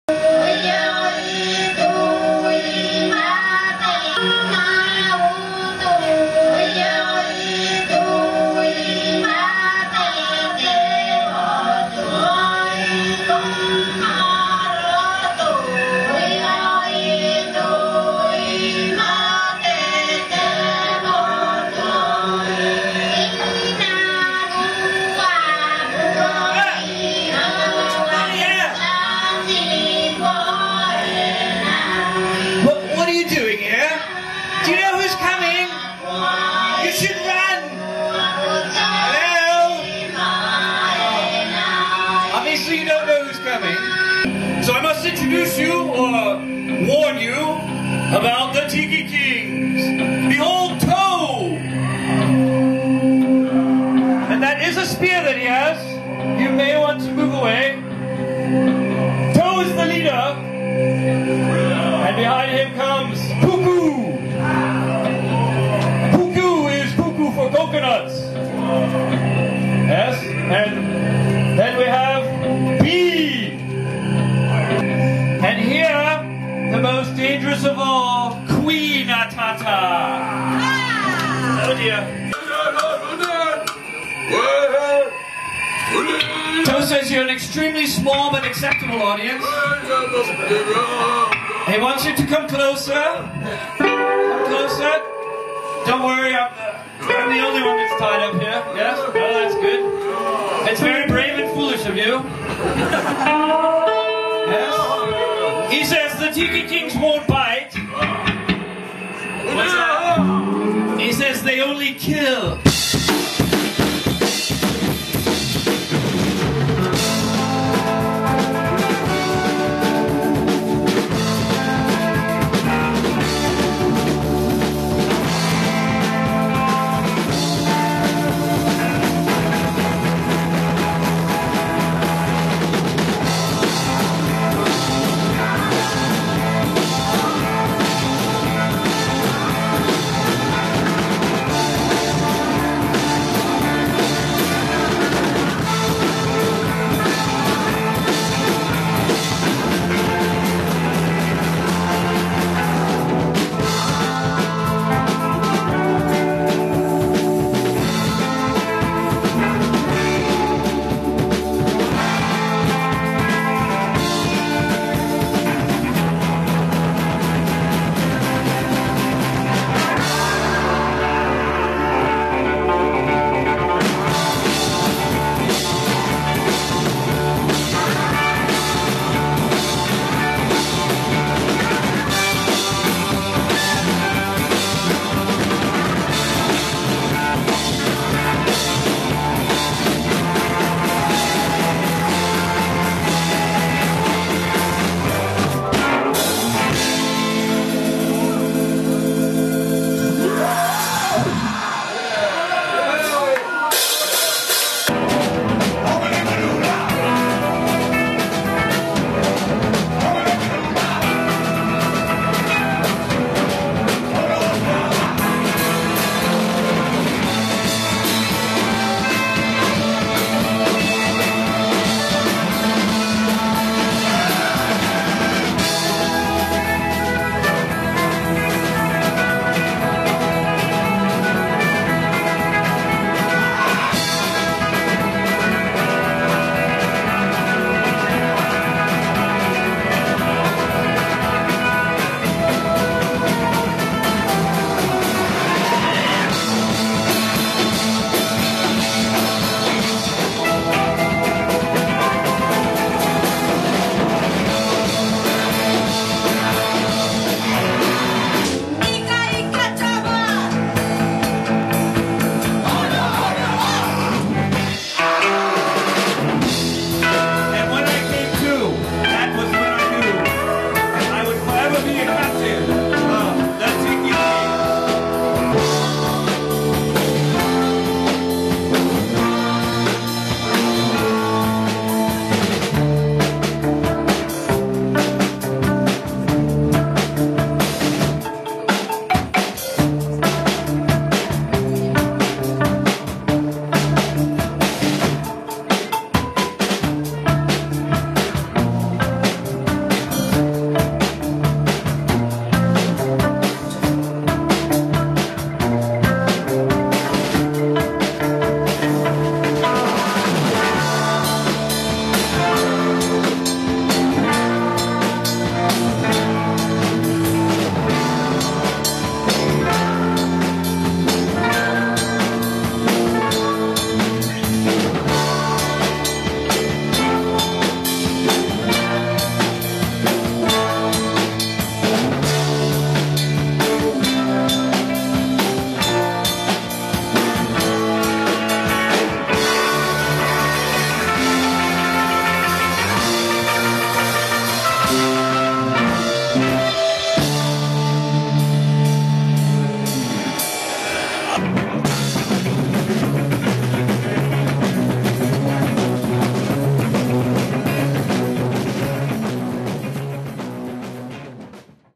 The Tiki Kings Do Ponyhof
Surf! Savage! Sacrifice! The Tiki Kings have once again left their South Pacific island to go on a wild rampage through the clubs. This time, they perform their tribal rituals at Frankfurt's Ponyhof.